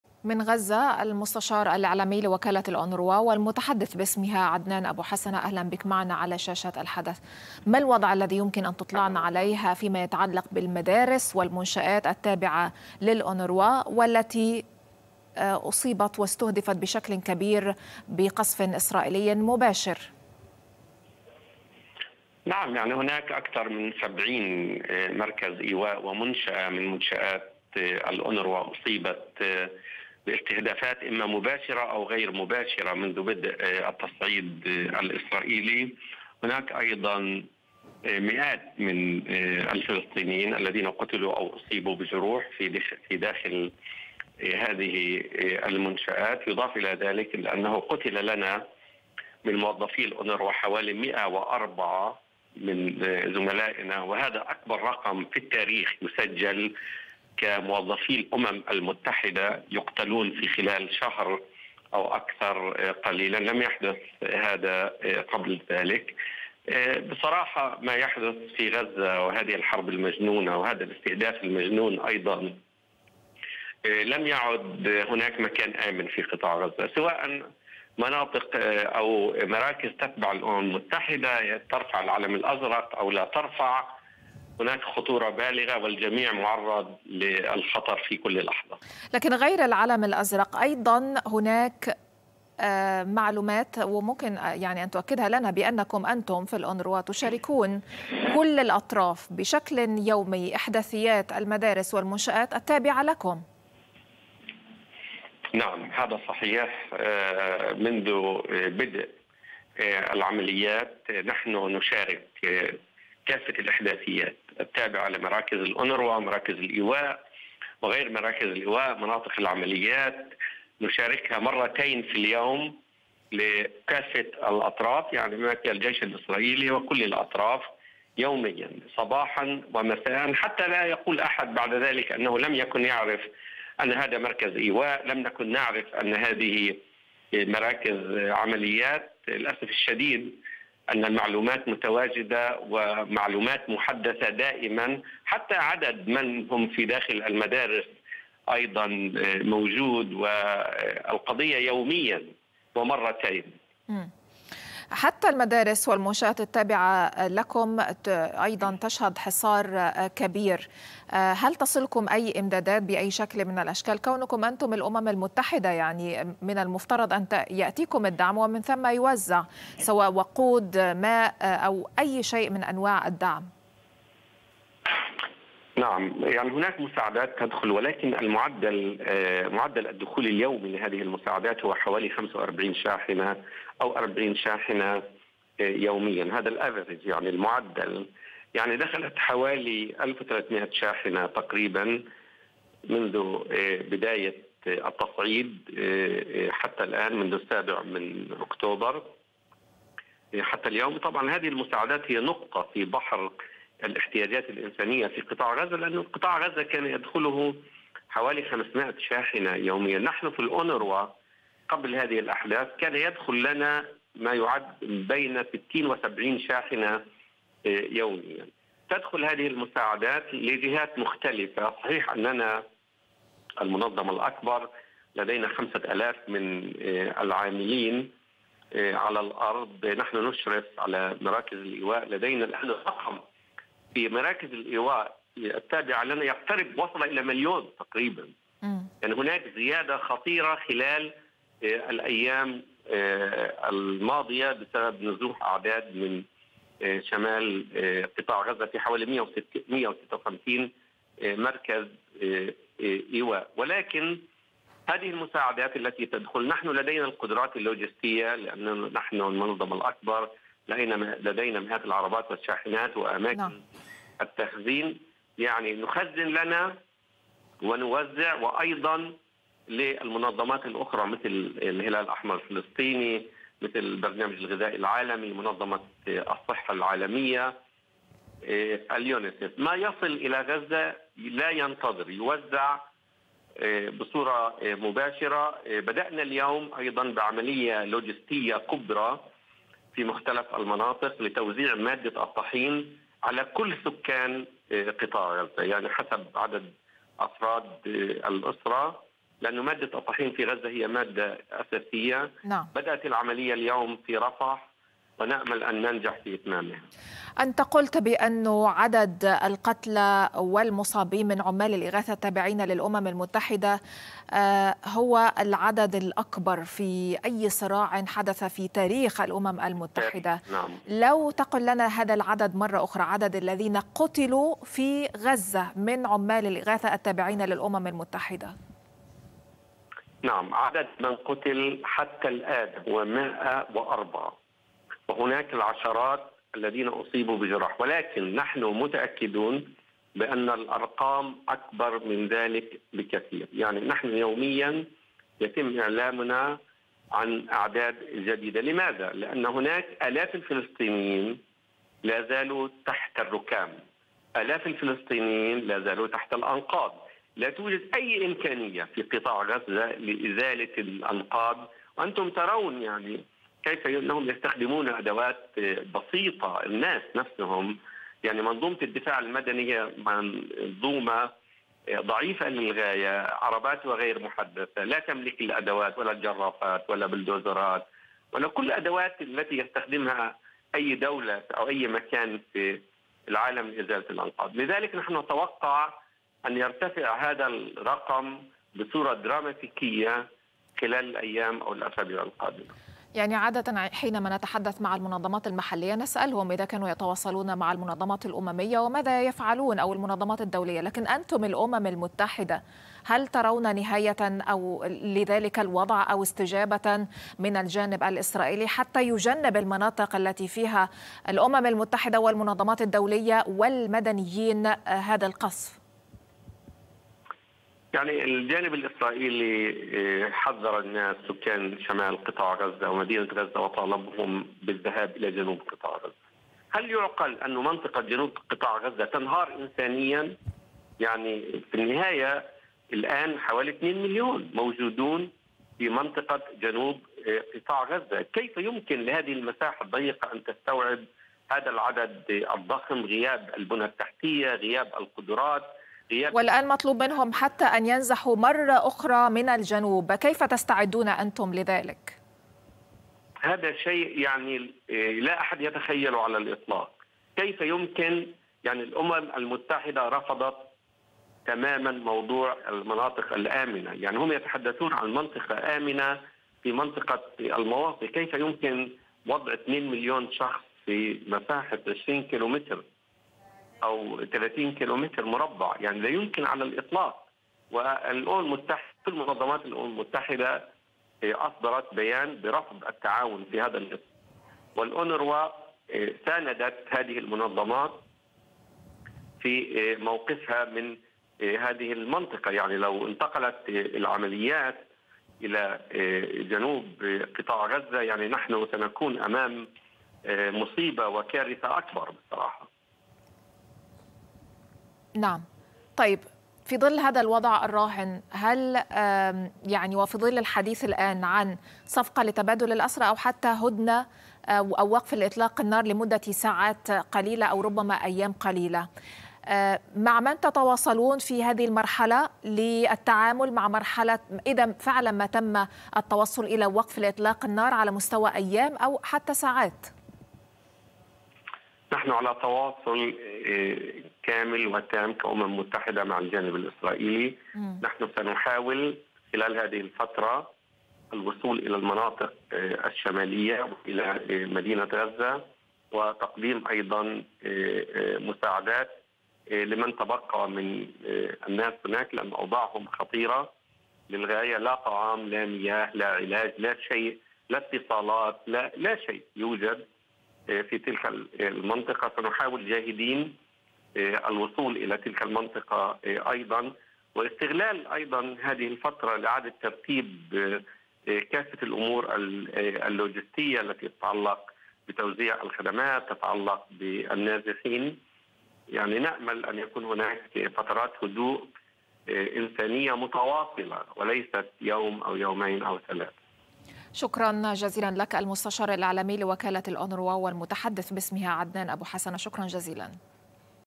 مقابلة خاصة لـ"الحدث" مع المتحدث باسم وكالة الأونروا عدنان أبو حسنة
المتحدث باسم وكالة الأونروا عدنان أبو حسنة في مقابلة مع الحدث: مقتل 104 من موظفينا منذ بدء الحرب في غزة و إسرائيل تتجاهل تحديثاتنا حول وضع المدارس ومن فيها وتقوم بقصفها